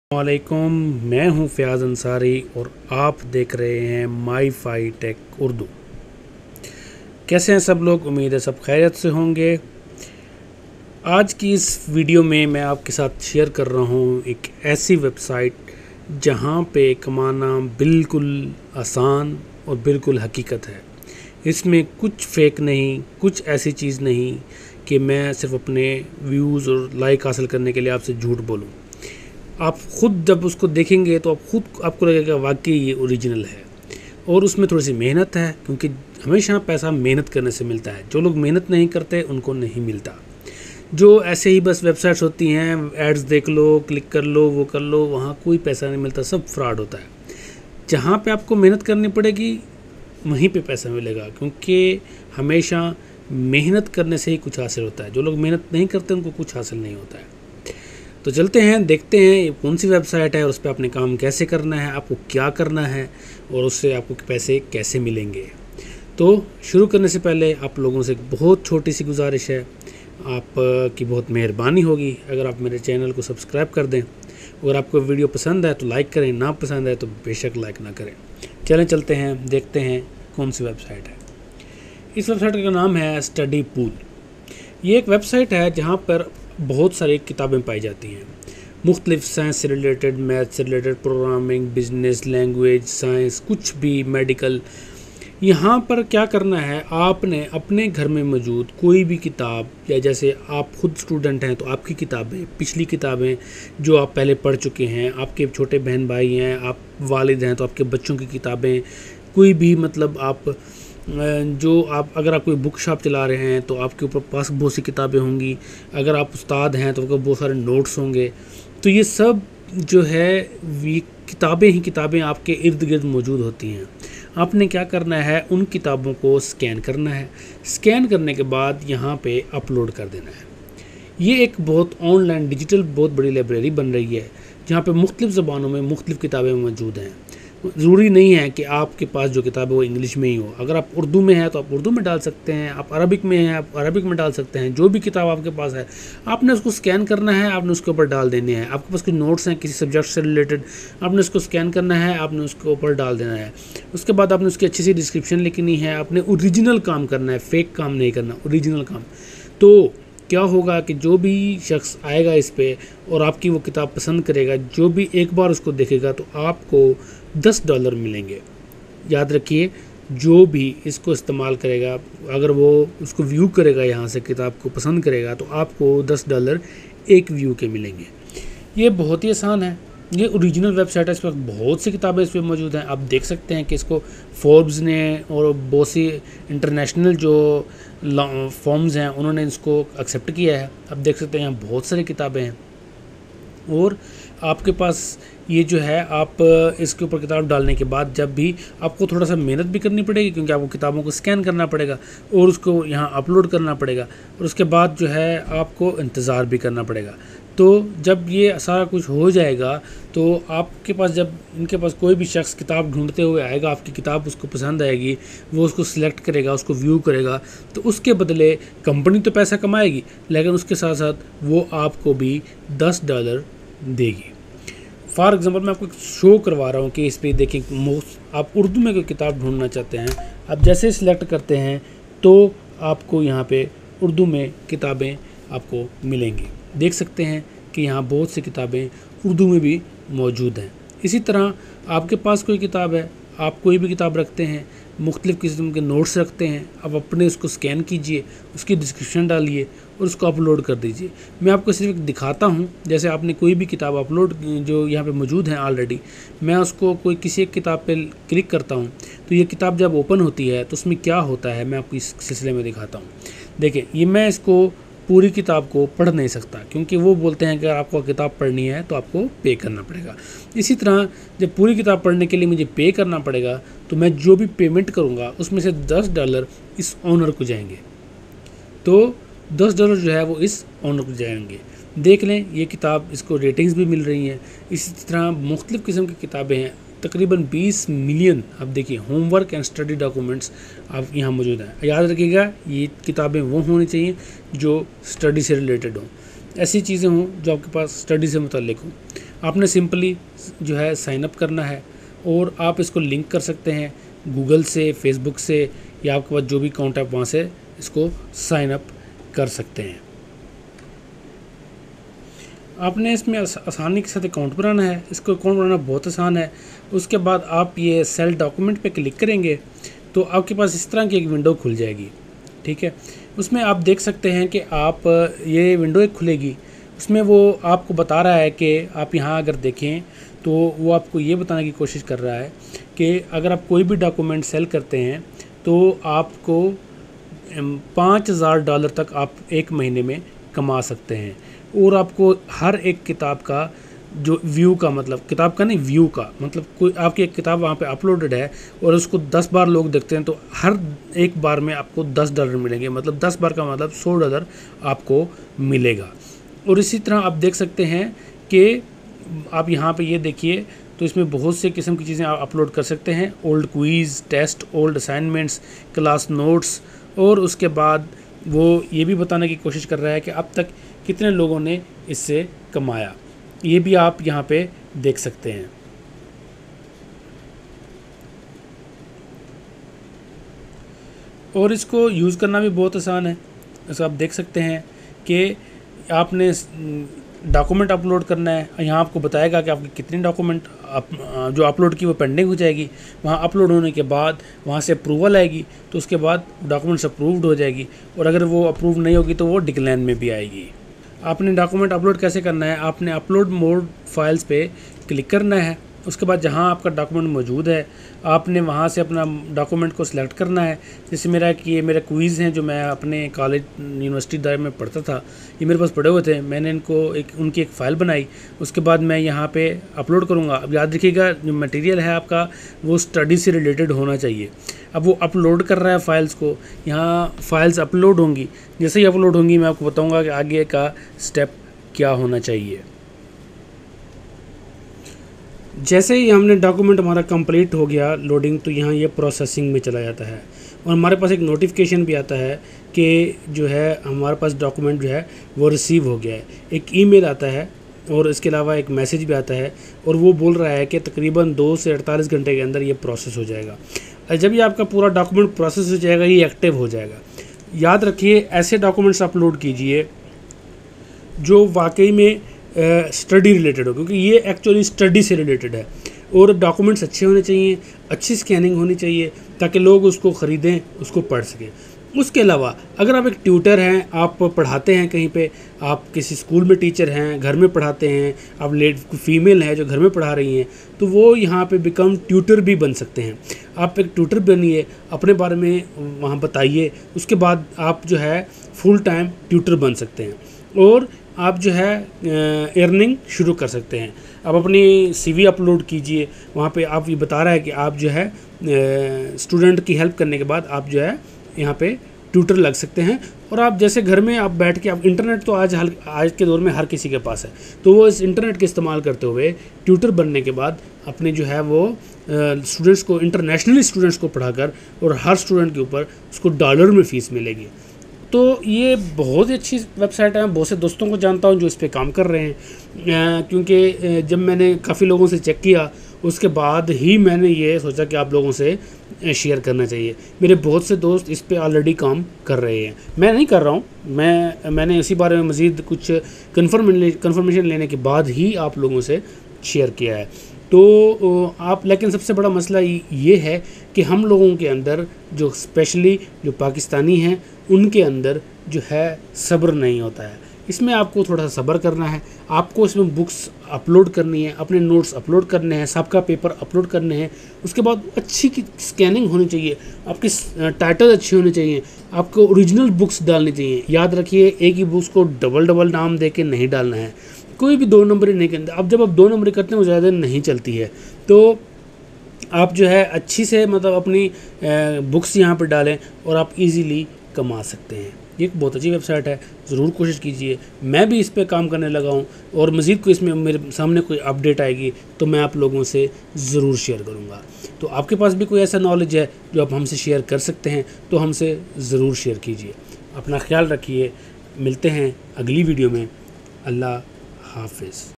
Scan and Sell Your Books, Papers, Notes, Handouts, & Earn Up to 5000$ Per Month | MYFI TECH URDU
Welcome to MYFI TECH URDU and I am Fiaz Ansari

if you are looking for 100% authentic website to earn money so this is for you and you can earn money up to 5000 $ by scanning and selling your own books, notes, exam papers, handouts, etc... you are a book seller scan books and earn money. this is not a fake website its 100% original website and give money.